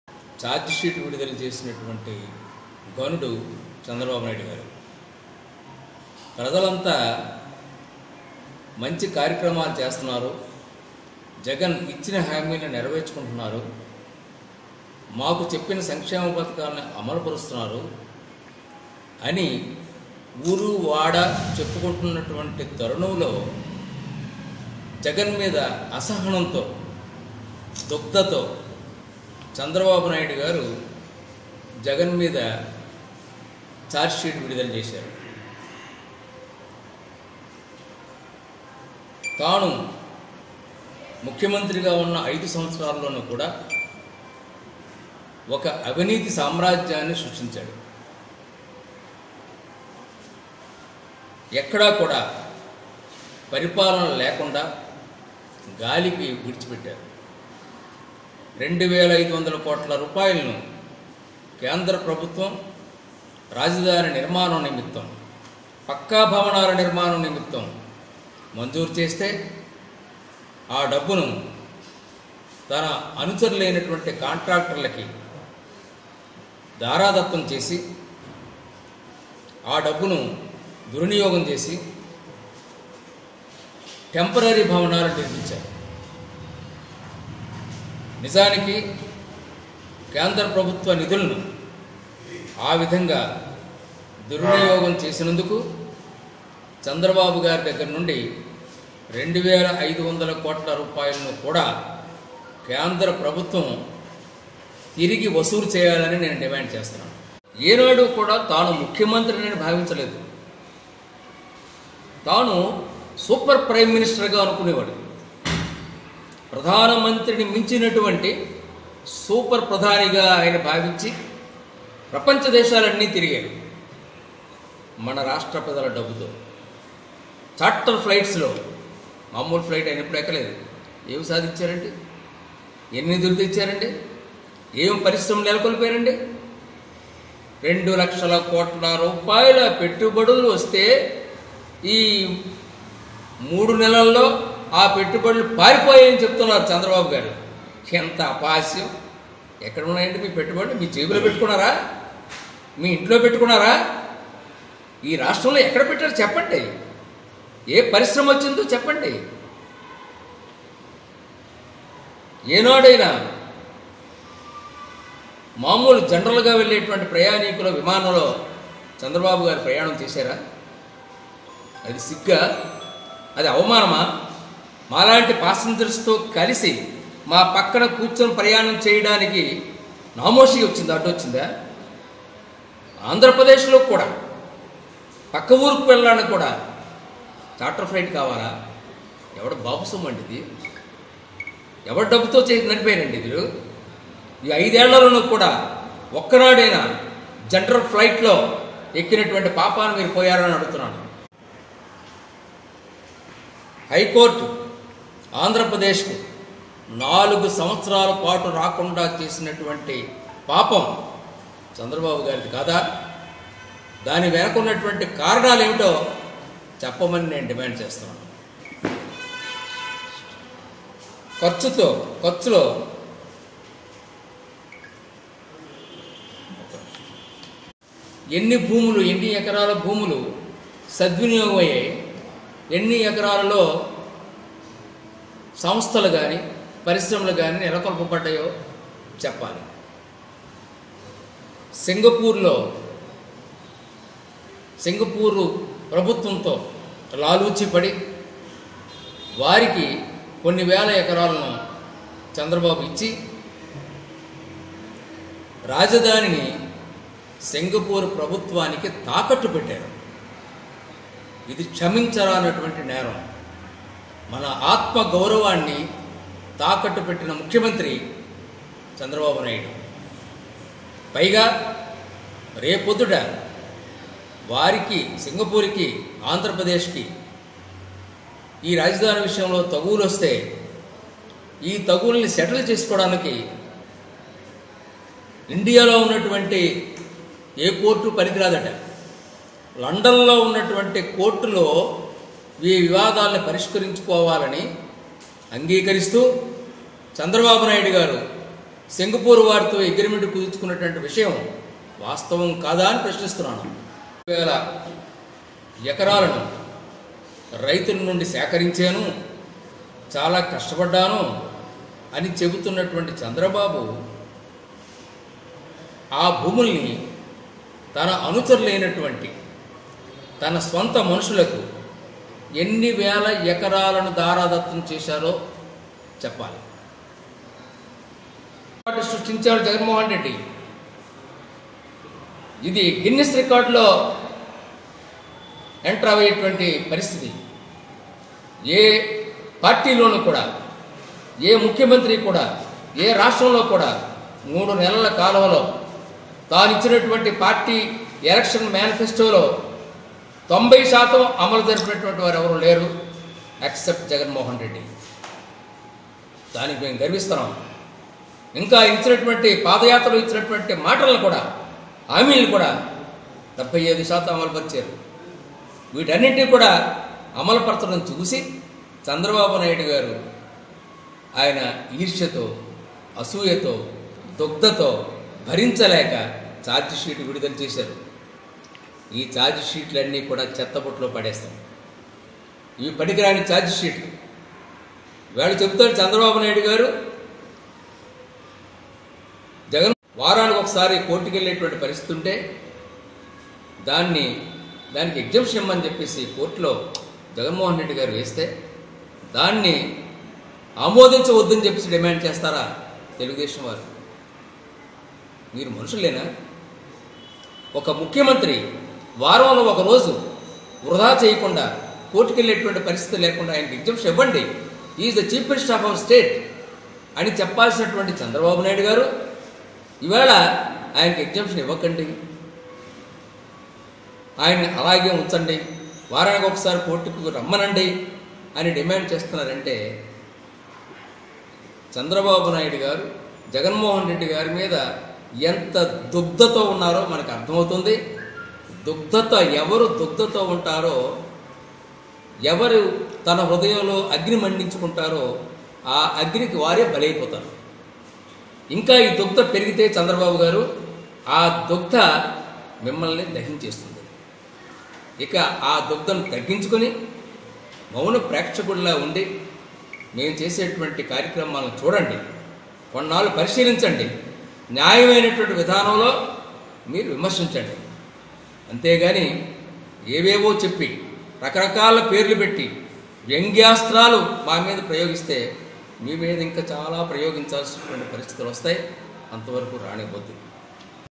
YSRCP Leader Dadi Veerabhadra Rao fire on Chandrababu on releasing charge sheet
YSRCP Leader Dadi Veerabhadra Rao fire on Chandrababu on releasing charge sheet - 9th Sep 2019